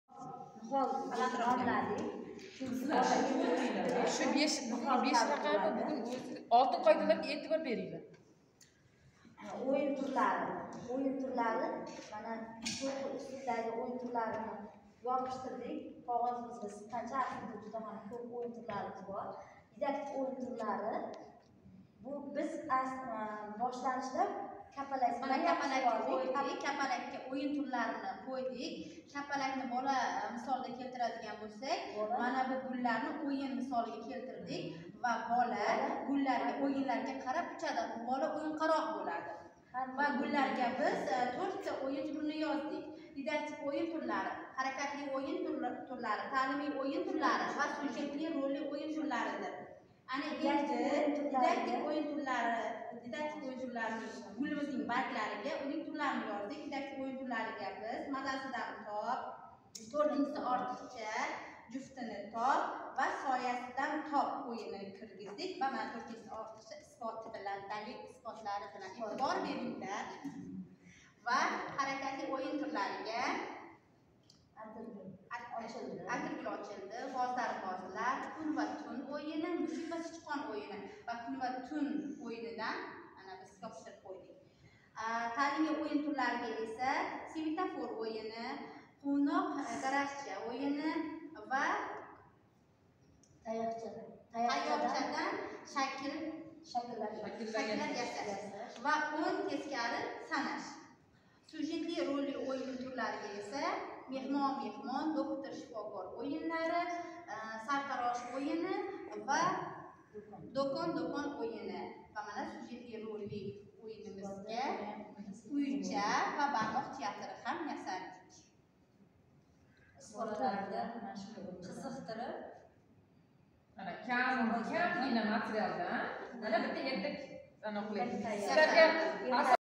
"Õyin orqali õrganish" mavzusi
🟢 Sirdaryo viloyati Sayxunobod tumanida Jaxon banki hamda Maktabgacha ta'lim agentligi hamkorligidagi Õzbekistonda erta bolalkidan rivojlanish"loyixasi asosida "Õyin orqali õrganish" mavzusidagi  maktabgacha ta'lim tashkilotlari pedagoglari uchun   40 soatlik Qisqa muddatli o'quv seminar-treninglarining  3-bosqichi tashkil etildi.  Ushbu treninglarda pedagoglarga trenerlar X.A.Djalolova va S.I.Kattabekova tomonidan erta yoshdan o'yin orqali bolaga ta'lim berish modulle bo'yicha amaliy va nazariy mashg‘ulotlar olib borilmoqda.

Делайте работу по дому. Благосостояние.